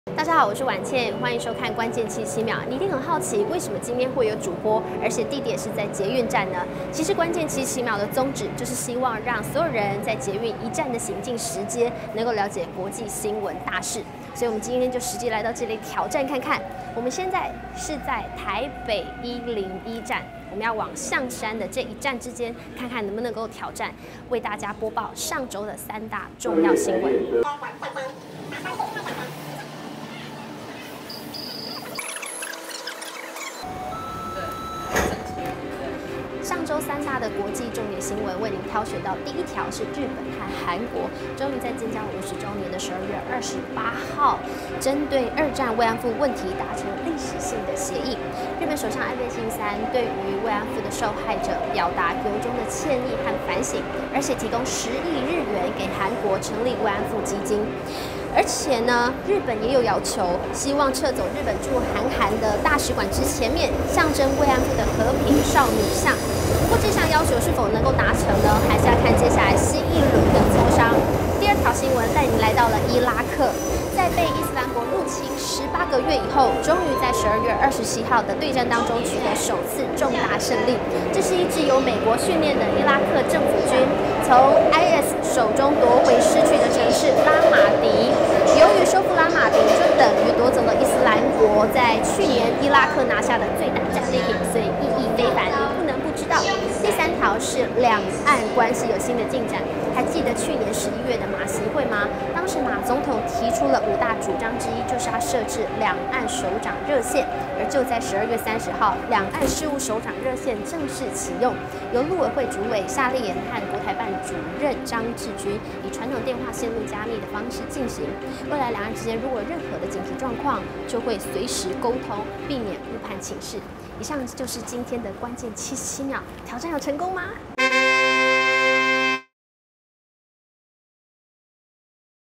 【關鍵77秒】1228~1231 一週三大新聞
一週(1228~1231)最重要的三大新聞：
☞日韓達成共識 道歉賠償慰安婦
☞伊拉克軍隊擊退IS 收復拉馬迪
☞馬習會後 兩岸熱線終於開通